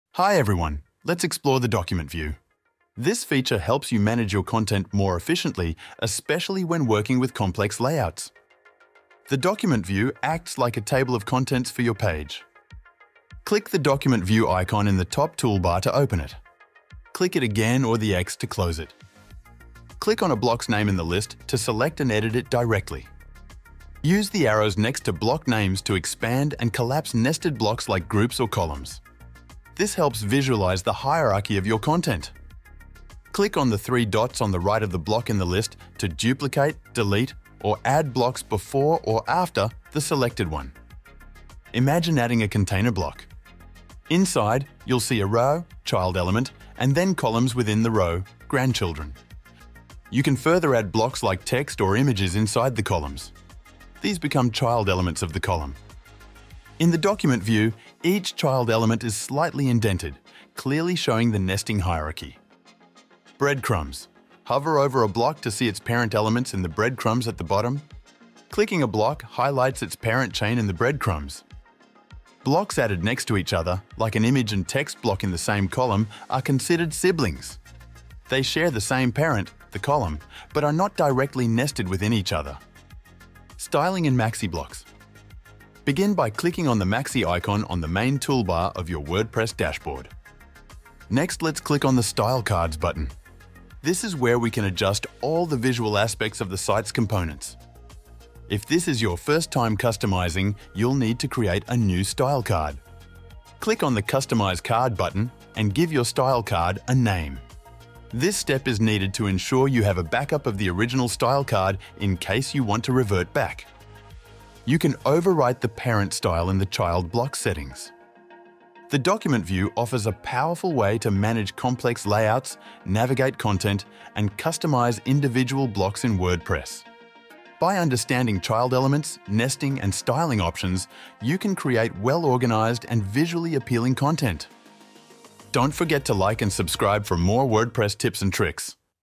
Element hierarchy WordPress document view
Mastering your content: a deep dive into the WordPress document view

Hey everyone, welcome back! Today, we're getting hands-on with WordPress and exploring a fantastic tool called the list view. This hidden gem helps you manage your content like a pro, especially when dealing with intricate layouts.

What is the document view?

Imagine a comprehensive table of contents for your web page. That's essentially what the list view does! It displays all the individual blocks used in your content, like paragraphs, images, headings, and more. This provides a crystal-clear overview of your content's structure and organization.

Using the document view like a boss

Opening and closing: Click the list view icon in the top toolbar to open or close it, just like opening and closing a navigation bar.

Selecting blocks: Want to edit something specific? Click on a block's name in the list to select and work on it directly.

Navigating nested blocks: Have complex layouts with groups or columns? Use the arrows next to block names to expand and collapse these nested sections. Think of it like zooming in and out on your content's hierarchy.

Basic actions: Right-click on a block in the list to duplicate, delete, or add new blocks before or after the selected one. It's like having a mini-control center right at your fingertips.

Understanding children and nesting

Think of adding a container block. Inside, you'll see a row child element and then columns within the row grandchildren. You can further add blocks like text or images inside the columns, which become child elements of the column itself.

Visualizing nesting in the list view:

Indentation: Each child element is slightly indented, creating a clear visual hierarchy.
Breadcrumbs: Hover over a block to see its parent elements at the bottom. Clicking a block even highlights its parent chain in the breadcrumbs, making navigation a breeze.

Sibling elements vs. child elements: What's the difference?

Blocks added side-by-side, like an image and text block in the same column, are considered siblings. They share the same parent the column but aren't directly nested within each other.

=== Description here ===